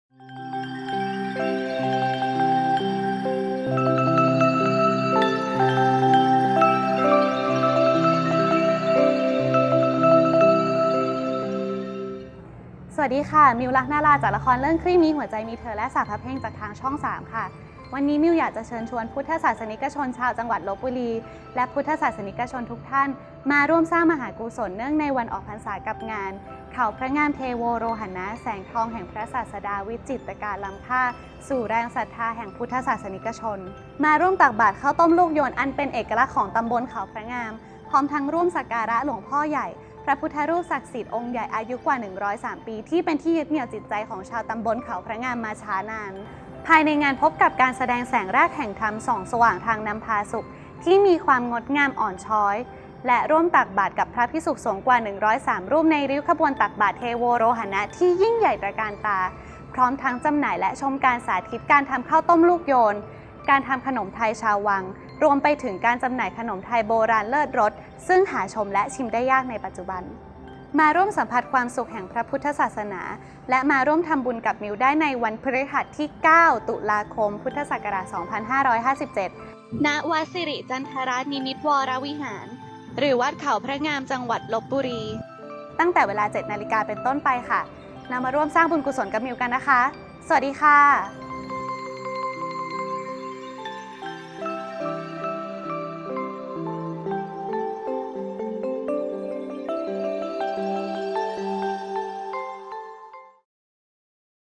ตักบาตรข้าวต้มลูกโยน เทศบาลเขาพระงาม
มาร่วมสร้างมหาบุญที่ยิ่งใหญ่ที่สุดในรอบปี กับงาน “เขาพระงาม เทโวโรหณะ” นมัสการหลวงพ่อใหญ่ ตักบาตรข้าวต้มลูกโยน นี้ได้ ในวันพฤหัสบดีที่ 9 ตุลาคม 2557 ณ วัดสิริจันทรนิมิตรวรวิหาร (วัดเขาพระงาม) ต.เขาพระงาม อ.เมือง จ.ลพบุรี เวลา 07.00 น.